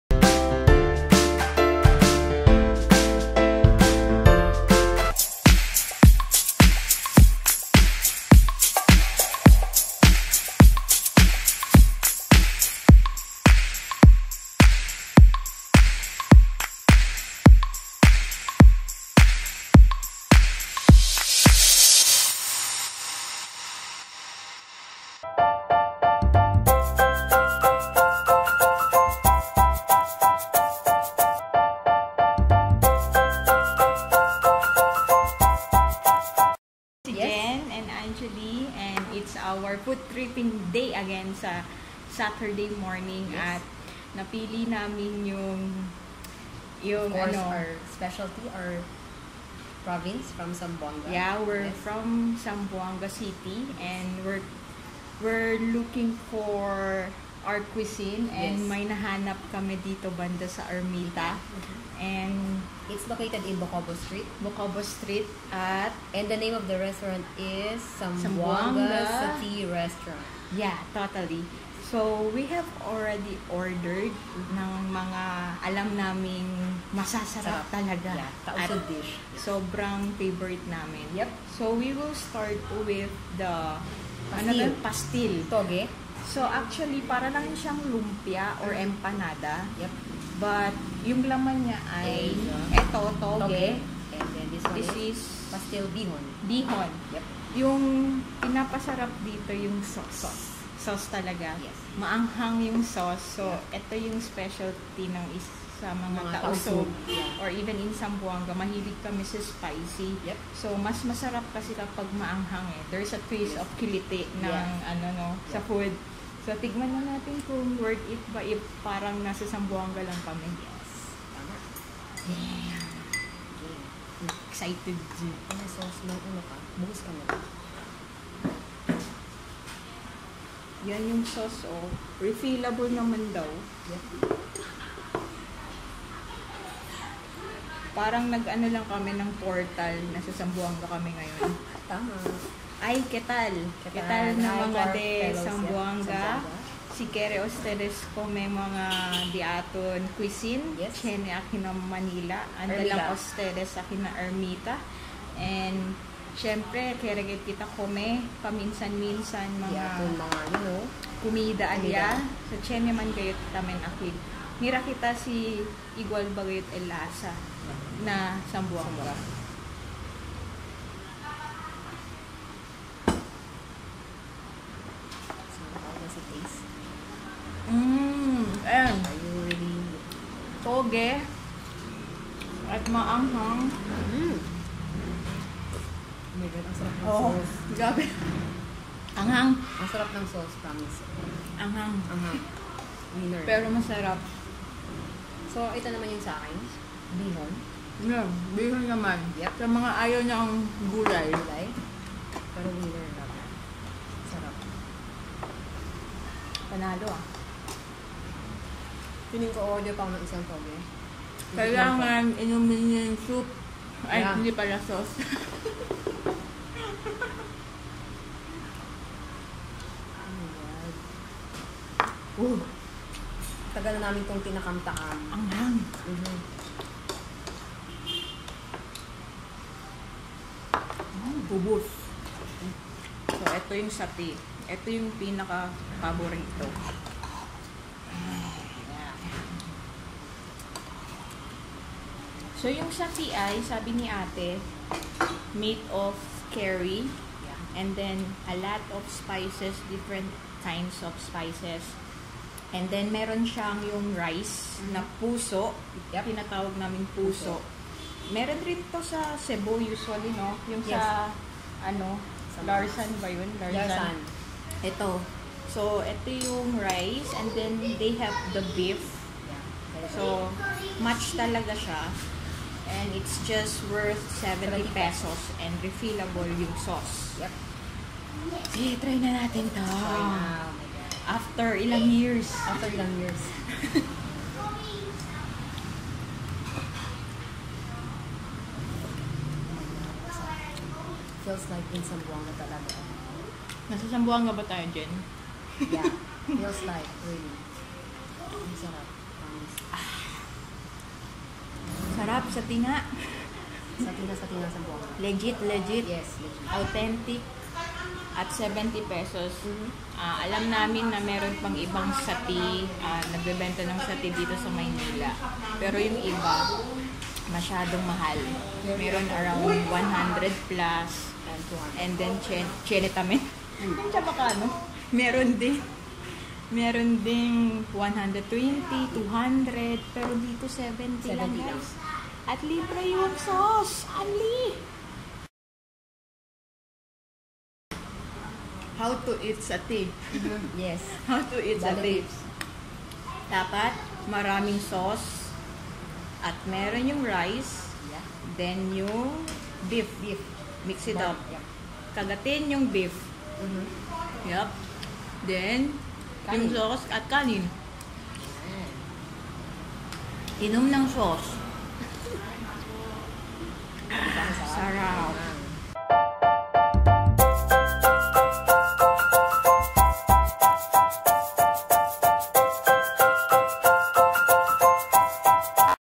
Food Review | Zamboanga Classic Satti | Malate | Part 1
In search for Tausug Cuisine in Metro Manila.

Restaurant Name:
 - Zamboanga Classic Satti Restaurant
Location:
 - 1678 Bocobo St, Malate, Manila, Metro Manila, Philippines

Foods we reviewed:
- Pastil (choices of toge and bihon)
- Satti (choices of beef and chicken)

Local delicacies in Mindanao specifically in Zamboanga City.

Tuned in for more Tausug Cuisine.